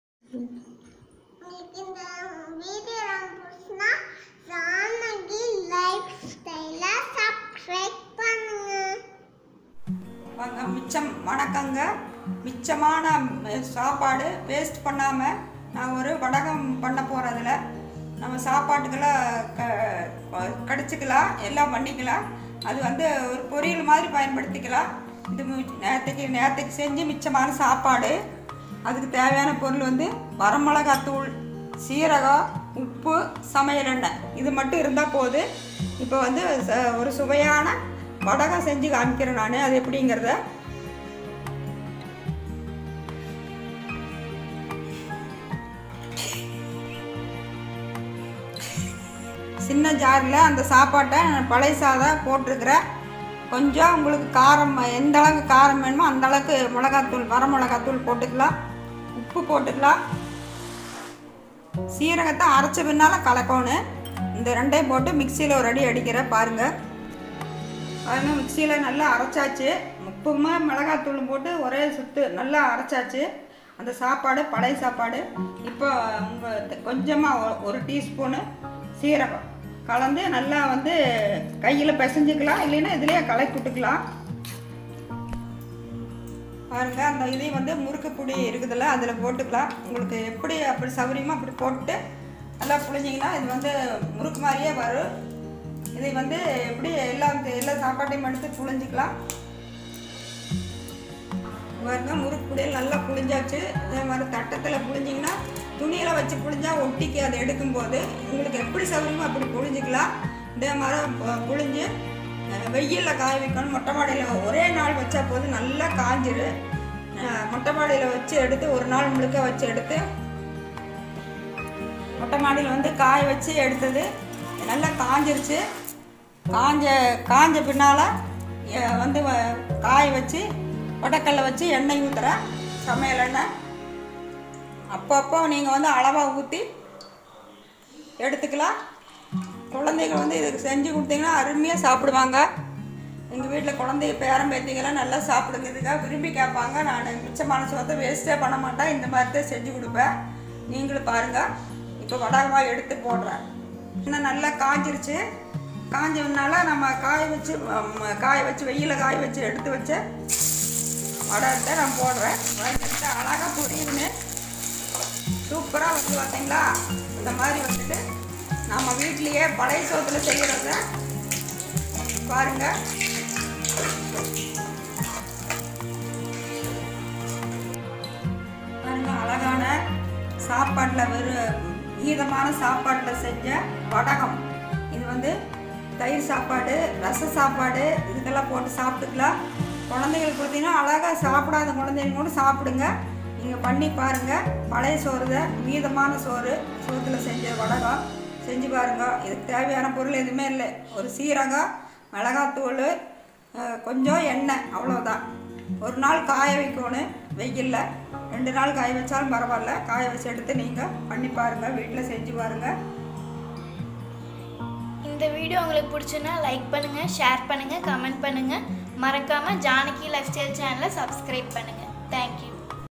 அரிசி வடகம்/ மீதமான சாதத்தில் வடகம் செய்வது எப்படி ? How to make Crispy vadagam/ Janaki Lifestyle!
Hi friends..here I have shared the recepie of crispy vadagam making at home...try pannunga..taste ah irukkum..babies will love it.. Arisi vadagAm is easy and simple to prepare